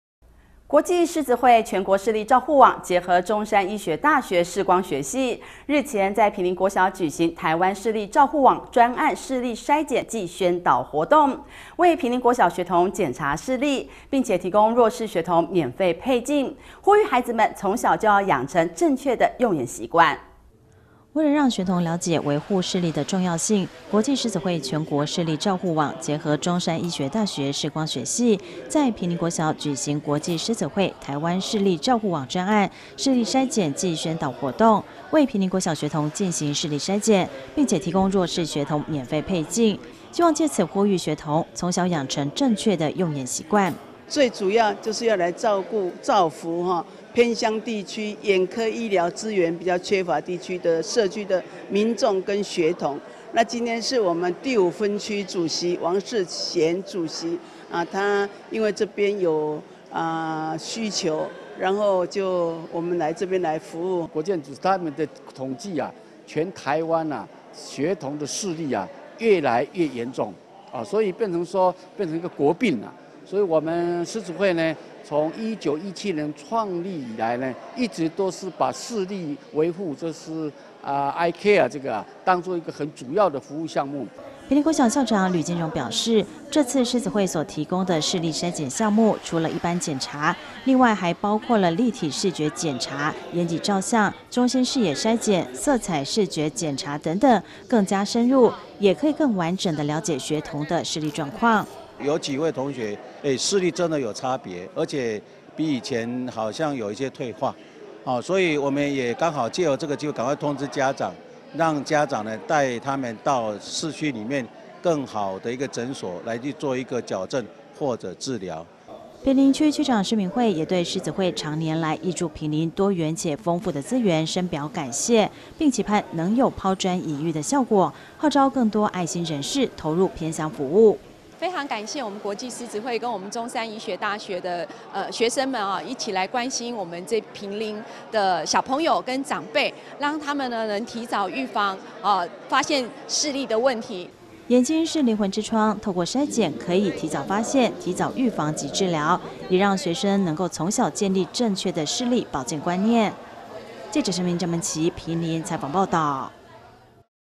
1090106【大新店地方新聞】關懷坪林學童視力 獅友提供篩檢
國際獅子會全國視力照護網結合中山醫學大學視光學系，日前在坪林國小舉行「臺灣視力照護網專案視力篩檢暨宣導活動」為坪林國小學童檢查視力，並提供弱勢學童免費配鏡，呼籲孩子們從小養成正確的用眼習慣。